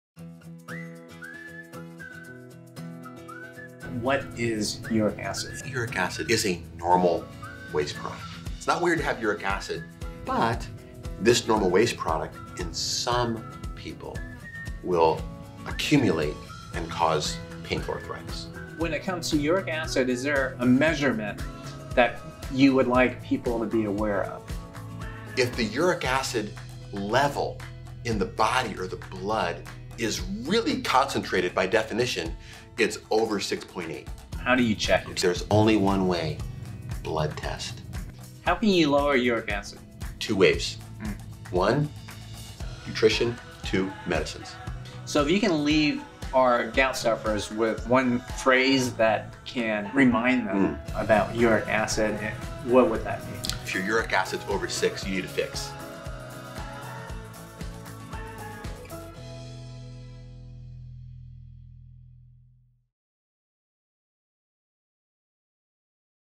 Uric Acid 101
Dr. Christopher Parker and Gary Ho discuss all things uric acid: how to check your levels, how to lower it, and more.